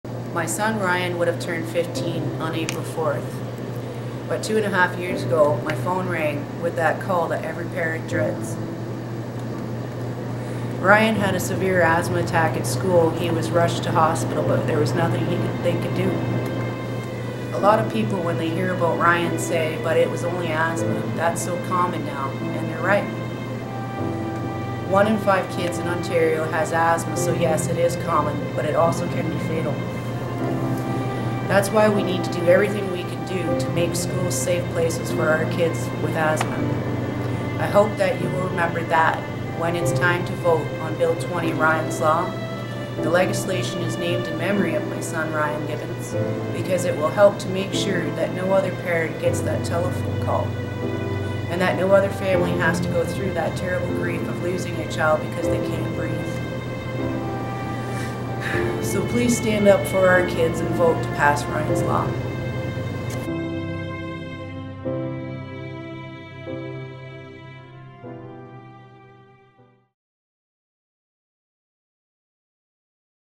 A Mother's Plea: Pass Ryan's Law
On October 9, 2012, 12-year-old Ryan Gibbons suffered an asthma attack at school. We lost Ryan because he was unable to reach his rescue medication in time to open his airways and allow him to breathe.

We are joining his mother Sandra in urging all Ontario MPPs to put their support behind Bill 20, Ryan’s Law. Doing so will help protect school-aged children living with asthma, and  ensure no more students suffer Ryan’s fate.